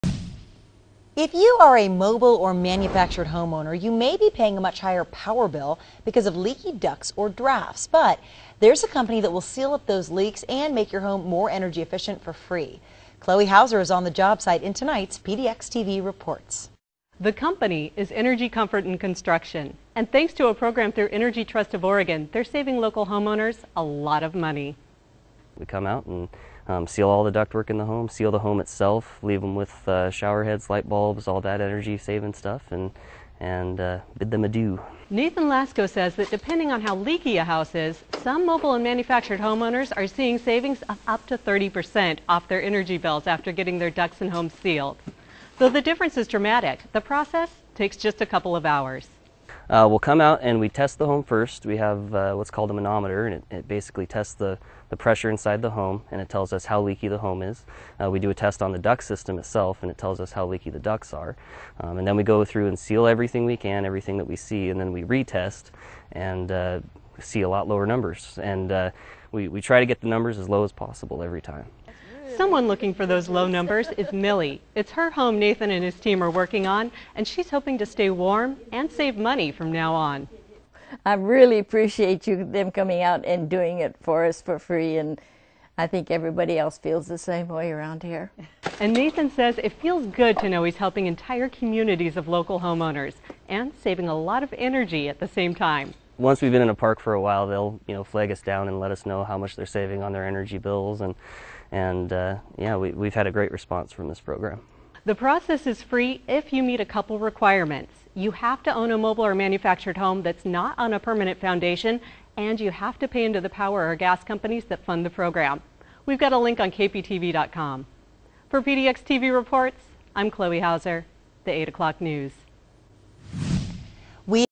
energy comfort
Chloe Houser of PDX TV in Portland OR learns about a program through Energy Trust of Oregon allowing mobile and manufactured homeowners to seal their homes and duct work for free!